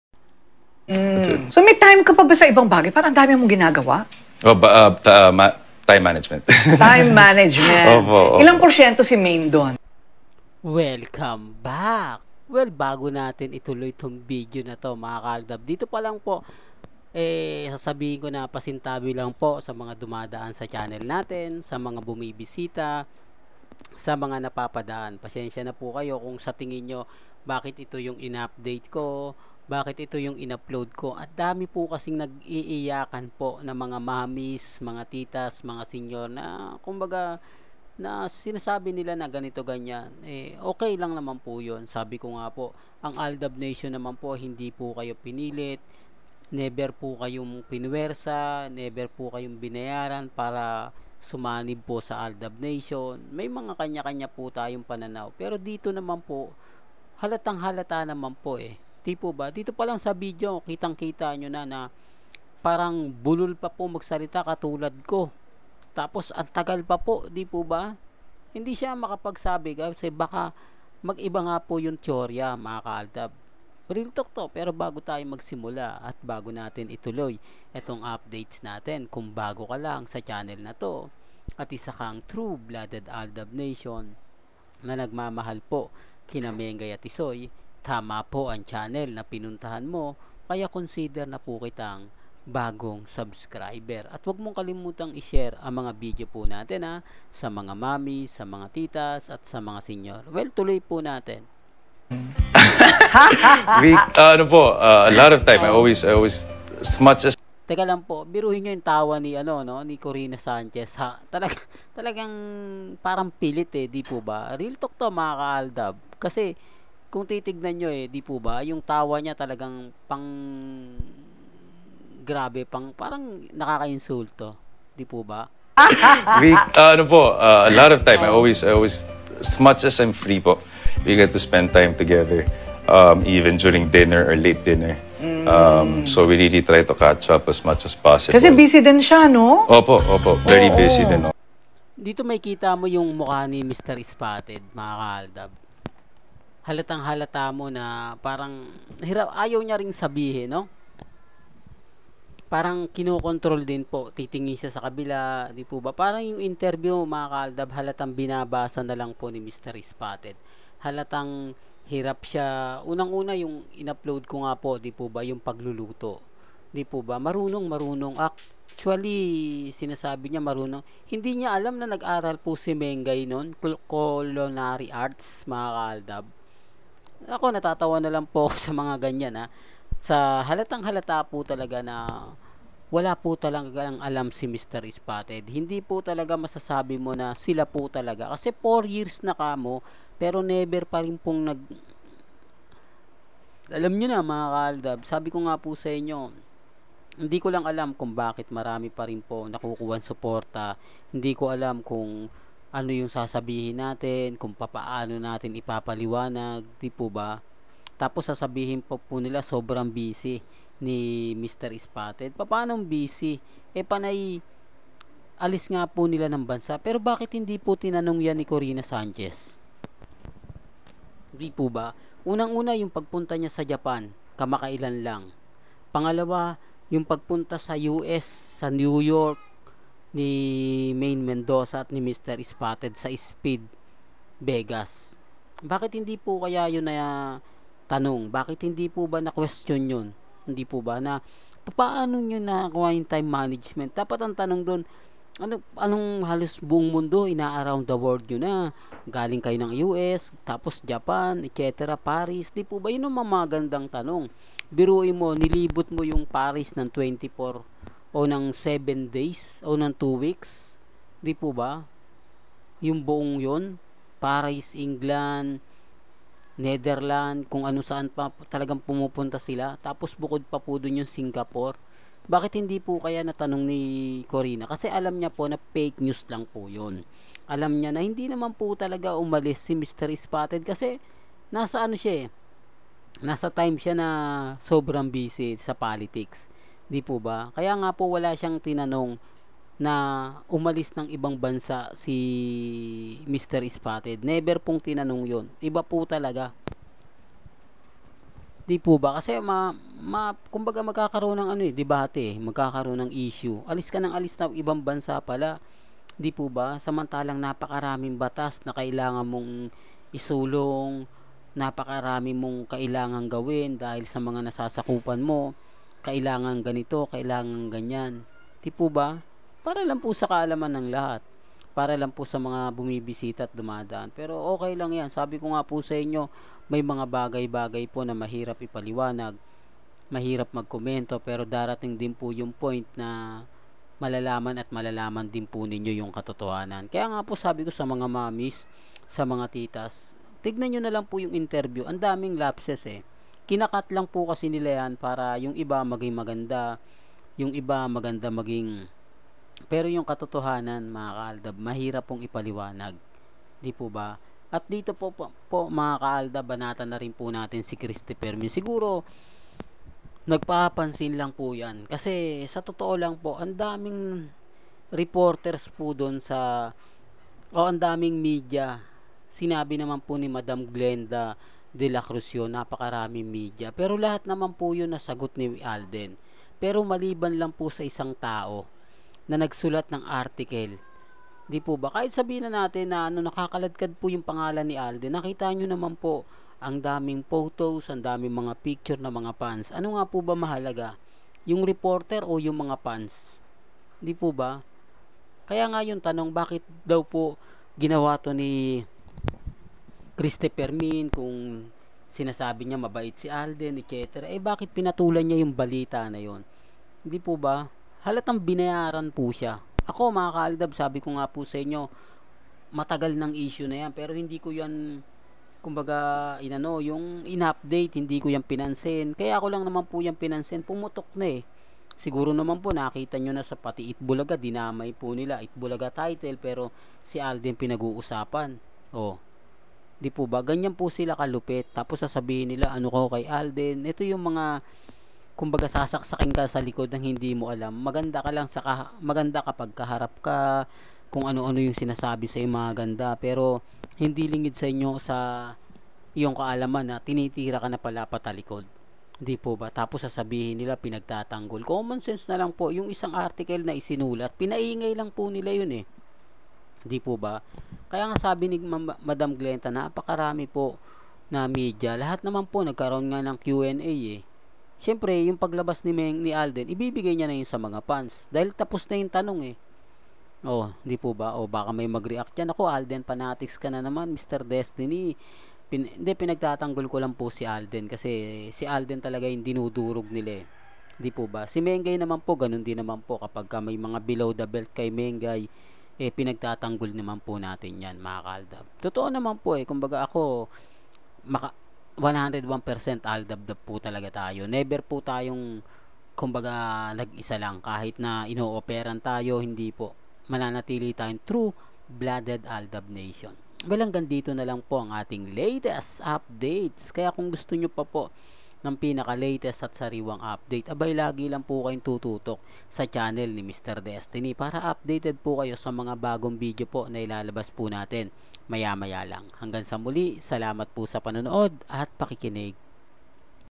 NEWS UPDATES Ayan po ang Resibo sa mga nagdududa pa rin sa Relasyon nila Maine Mendoza & Alden Richa
NEWS UPDATES Ayan po ang Resibo sa mga nagdududa pa rin sa Relasyon nila Maine Mendoza & Alden Richards